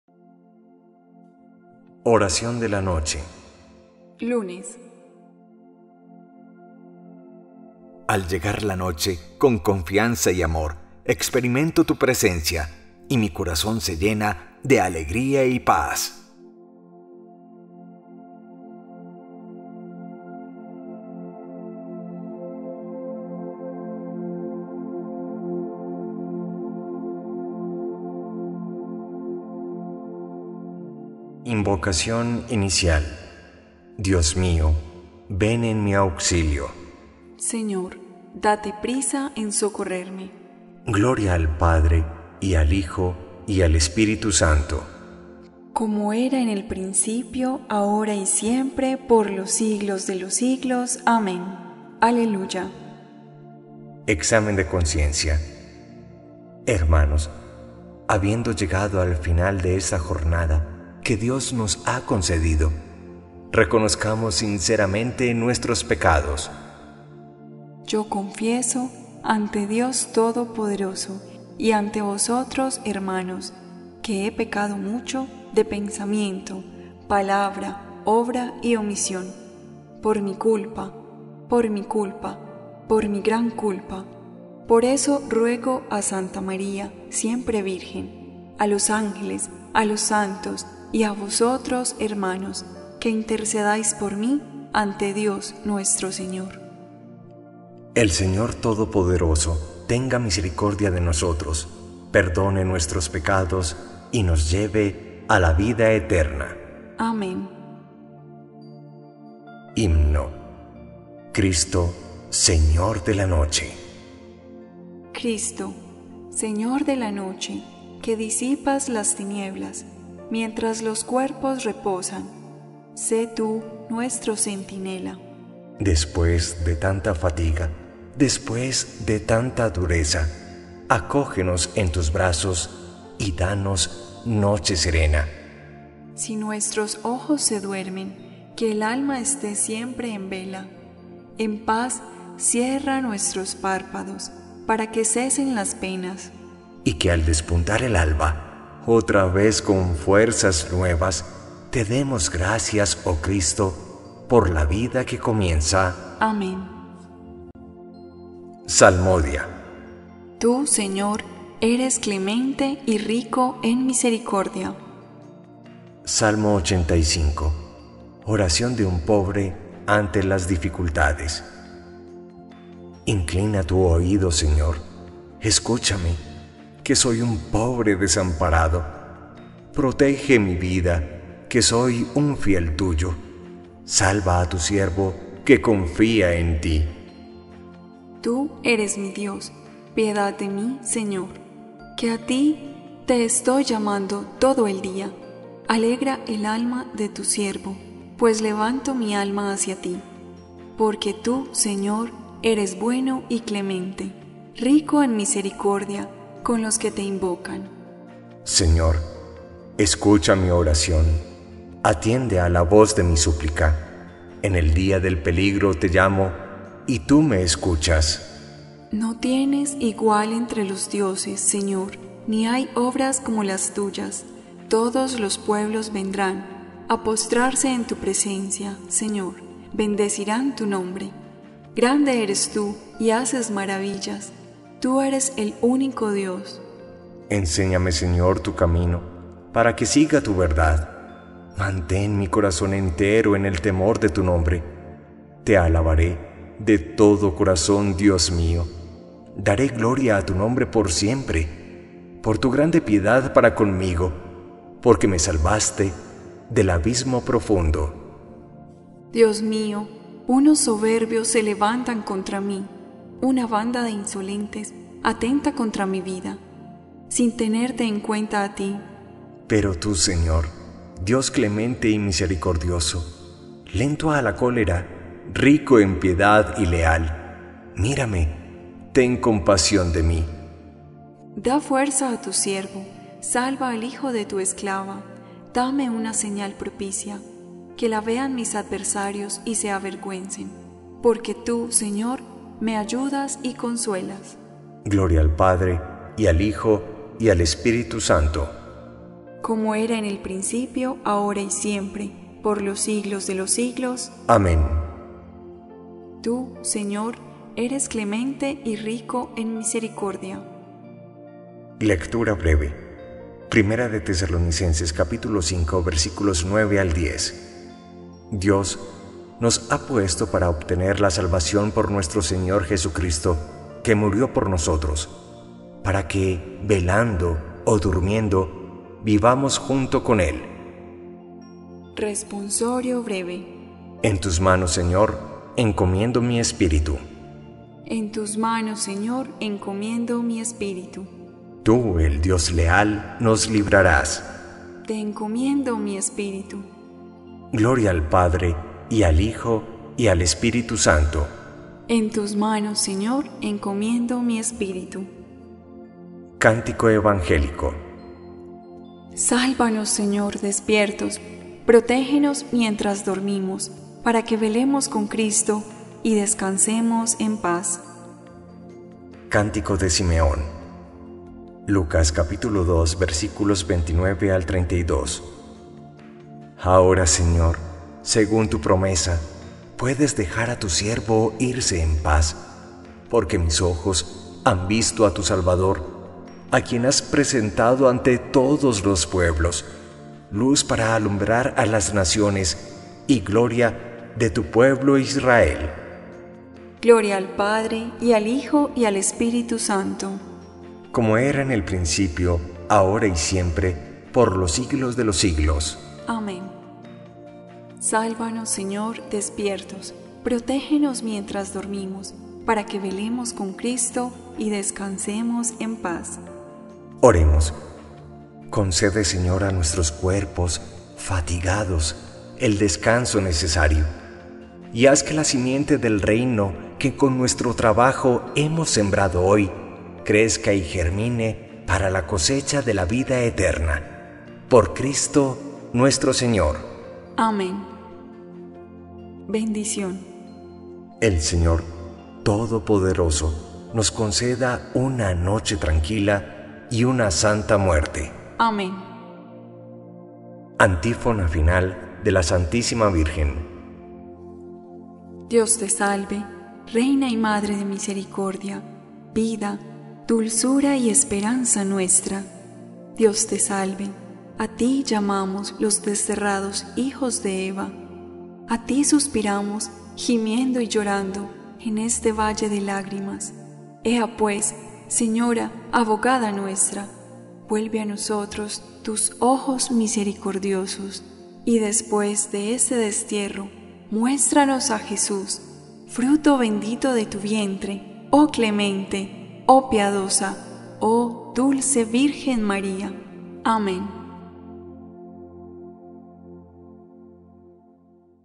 Oración De La Noche Lunes 18 Enero 2021 Padre Carlos Yepes. Completas
AMEN COMUNICACIONES y Padre Carlos Yepes
Te invitan a orar:

LA ORACION DE LA NOCHE: COMPLETAS

La"Oración de la noche" o "Completas" se hace
diariamente.
Unete con Nosostros  a Orar  diariamente

𝗘𝗡𝗖𝗨𝗘𝗡𝗧𝗥𝗔𝗡𝗢𝗦 𝗘𝗡